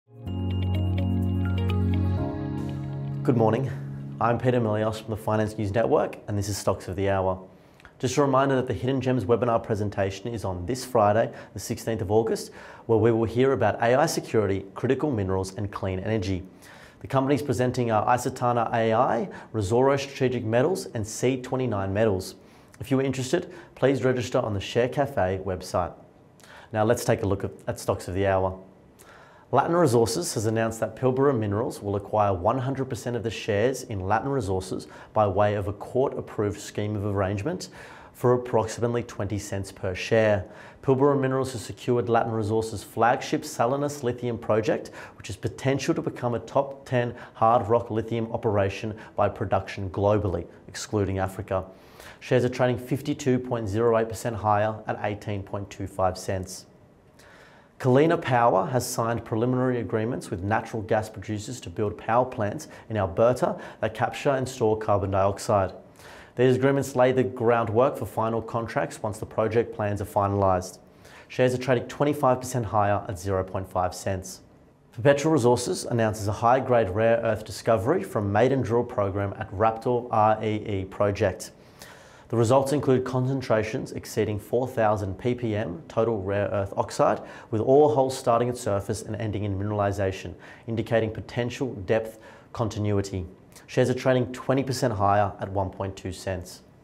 Stocks of the Hour: Latin Resources, KALiNA Power, Perpetual Resources
A snapshot of the stocks on the move, featuring Latin Resources (ASX:LRS), KALiNA Power (ASX:KPO) and Perpetual Resources (ASX:PEC).